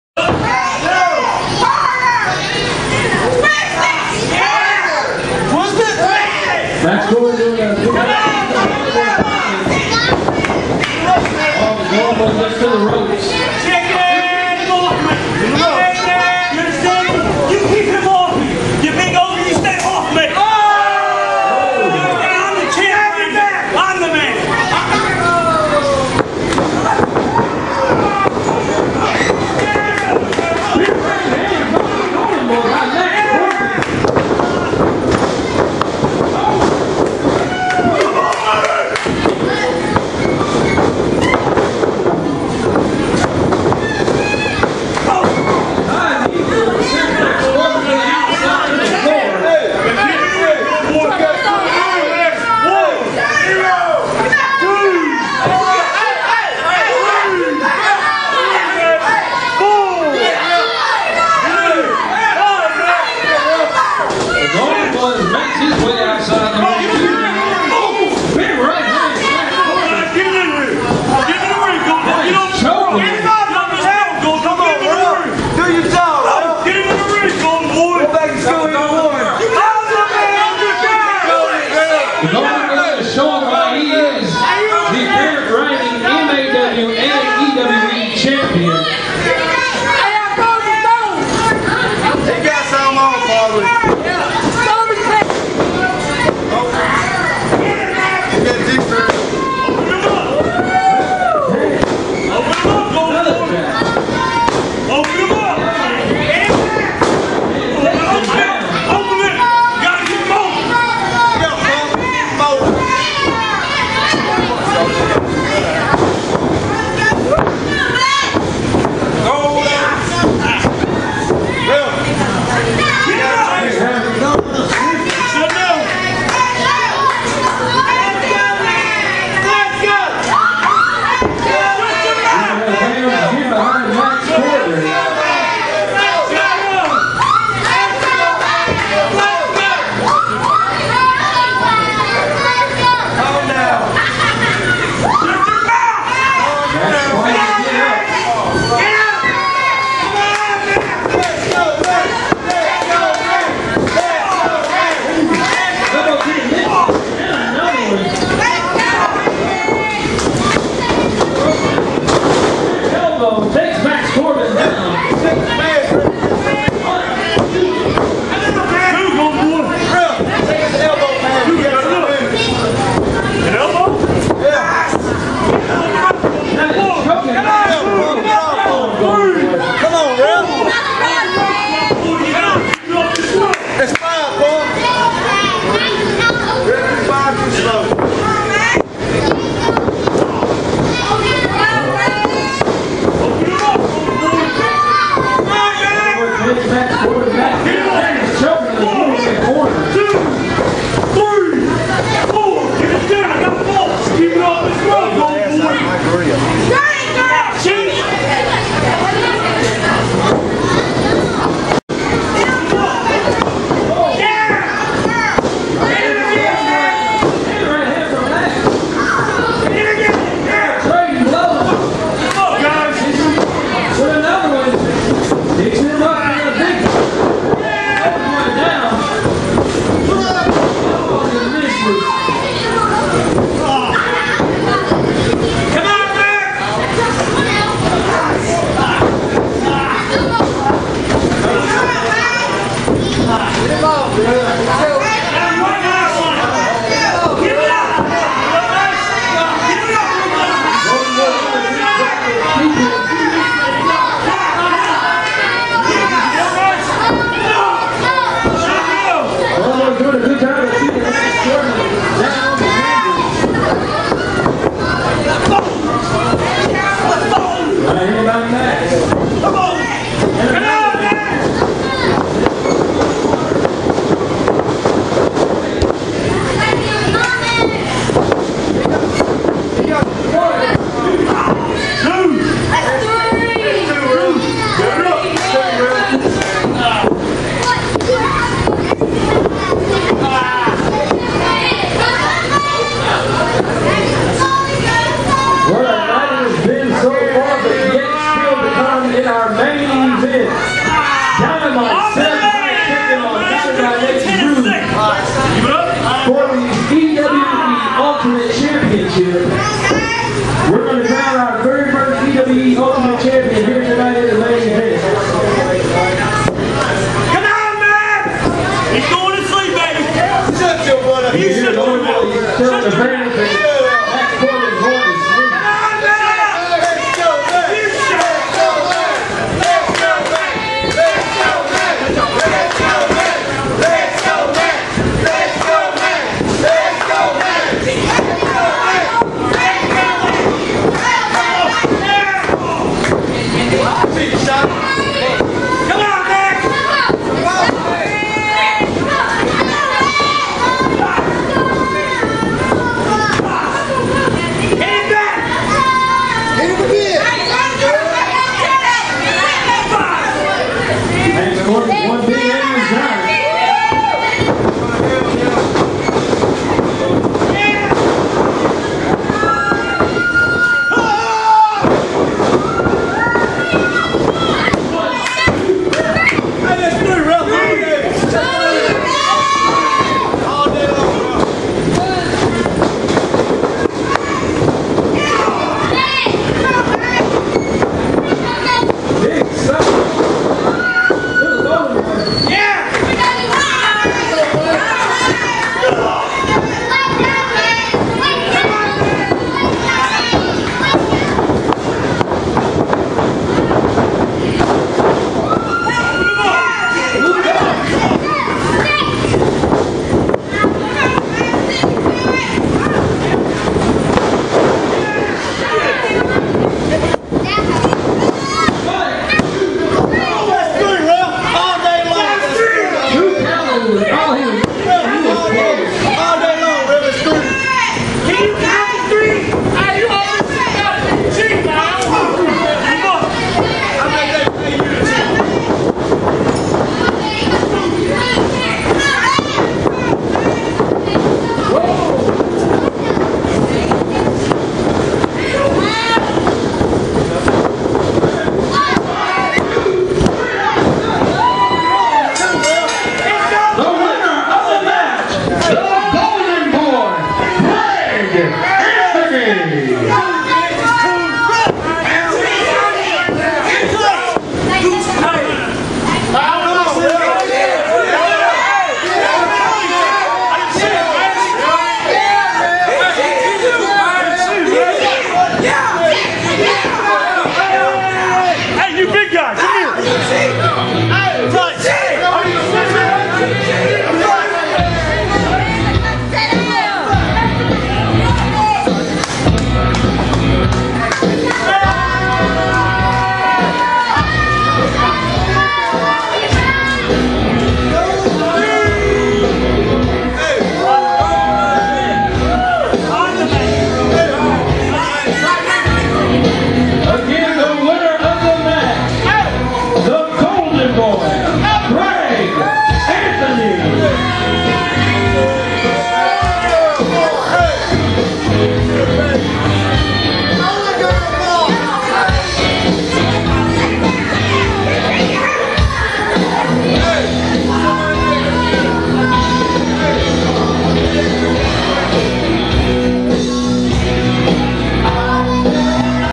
"The Golden Boy" Greg Anthony vs Maxx Corbin 2/2
The newly crowned Elite Wrestling Entertainment Heavyweight Champion, Greg Anthony goes one on one with 6'4", 255lb Maxx Corbin